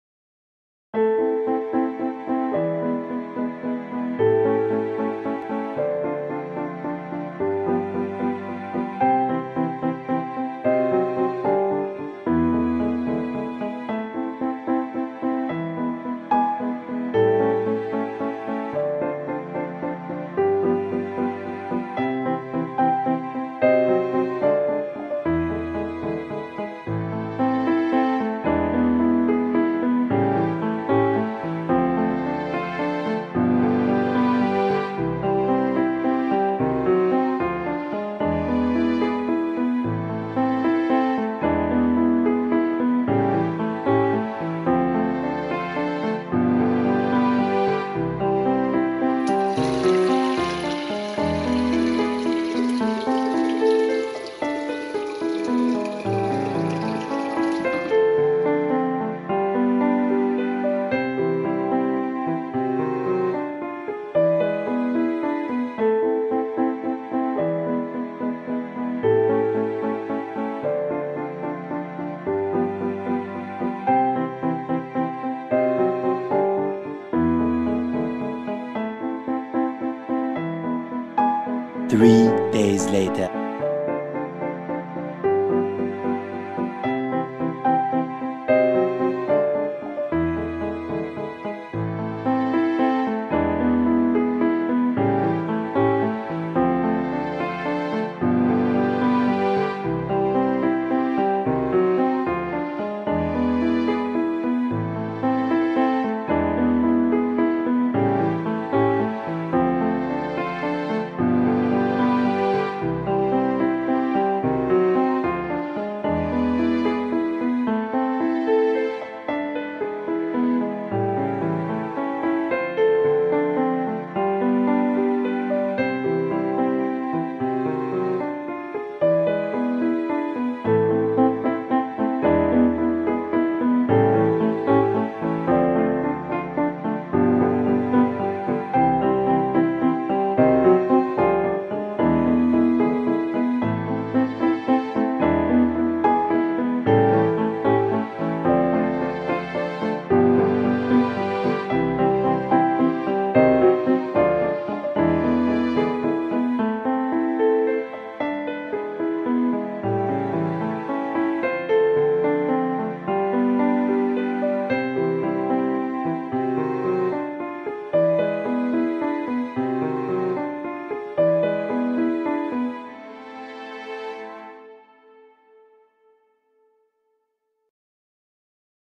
How to grow *FREE* food to feed your goldfish FOREVER! easy
In this video I show how I made a duckweed farm to feed a goldfish.
Goldfish need at least 20 gallons of space if kept in a tank, my goldfish was only in the tank temporarily and was then returned to its pond.